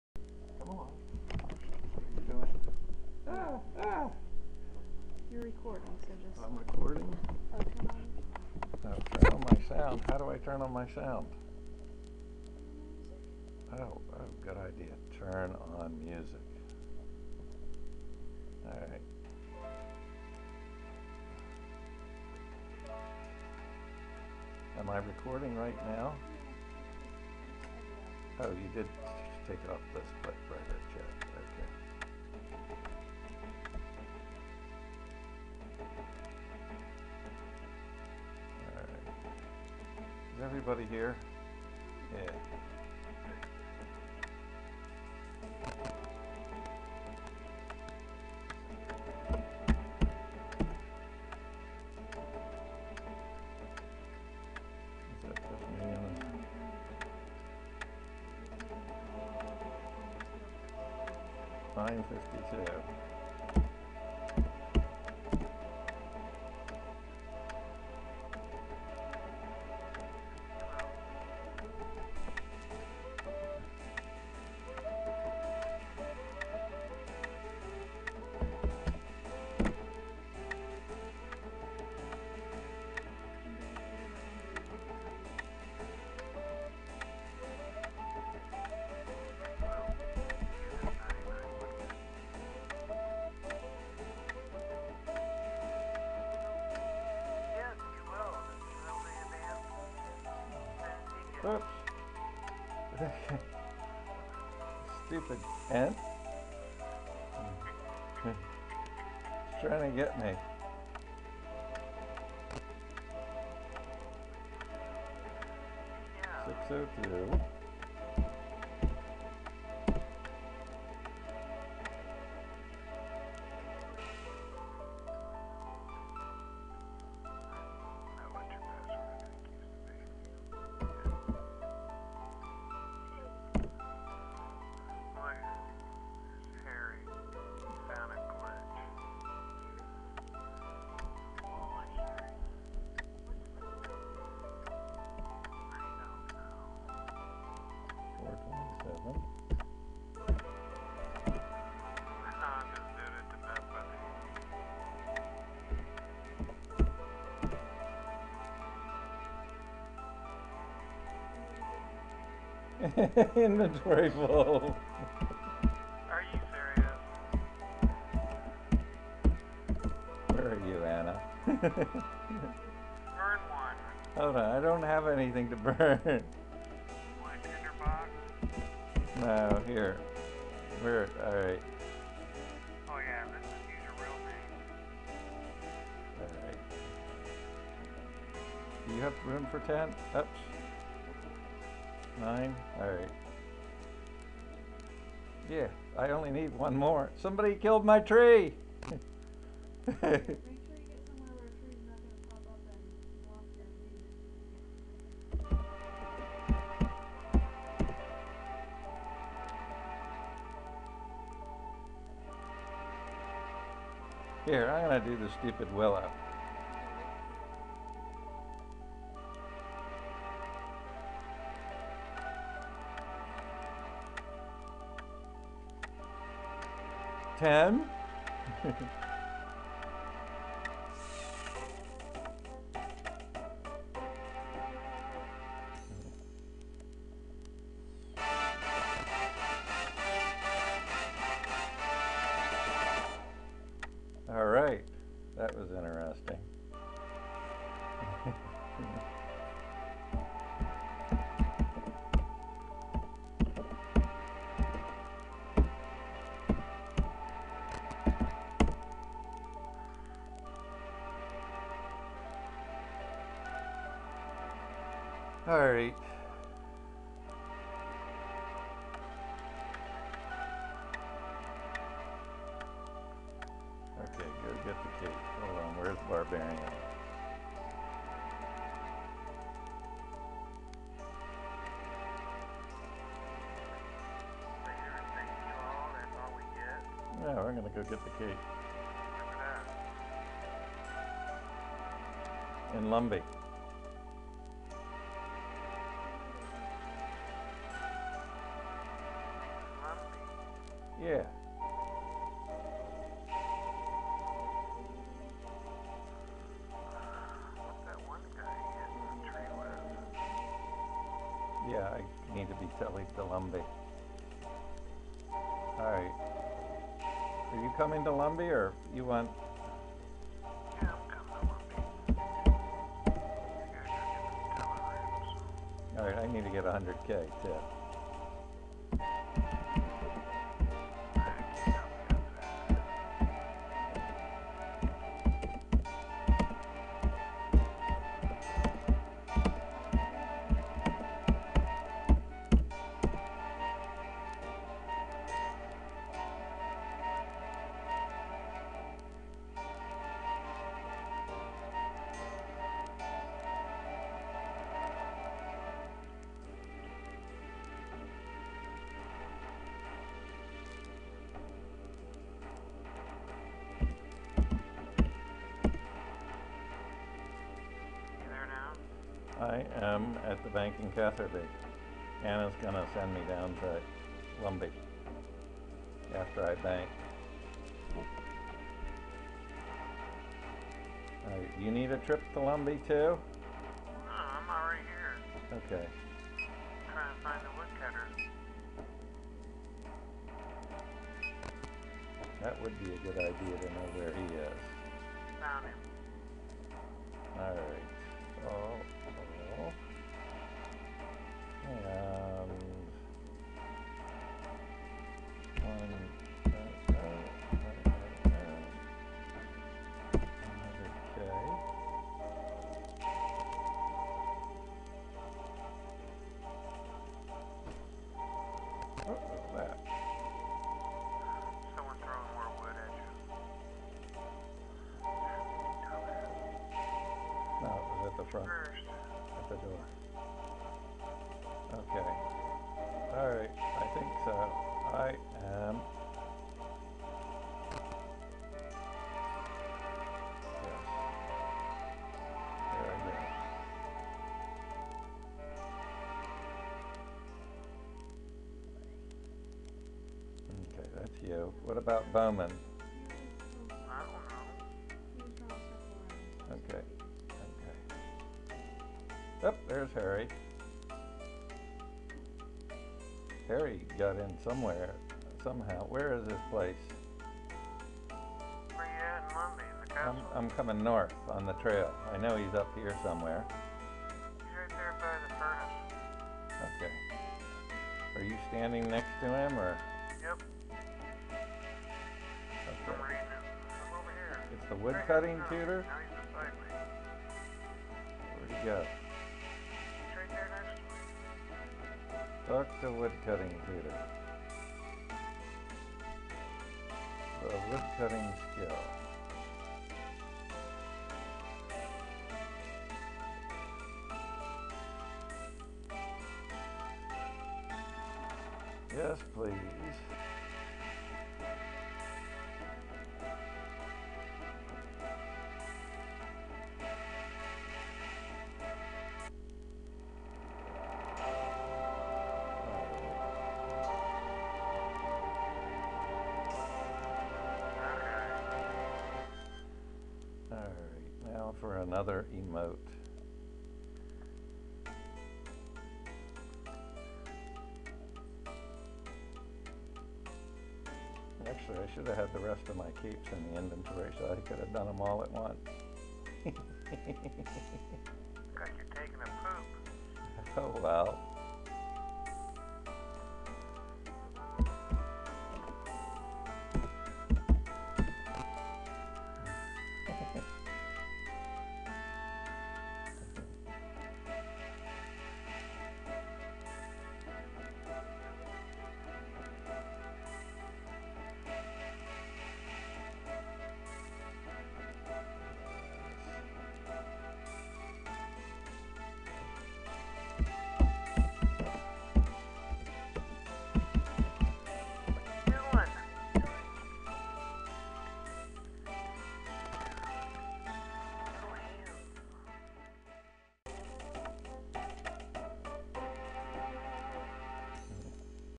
Homer3632 Gets 99 Woodcutting
We didn't realize it was recording our voices until watching it later. Enjoy!

You may need to turn up the volume to hear it... just don't forget to turn it back down afterward. I wouldn't want to be responsible for any ruptured tympanic membranes!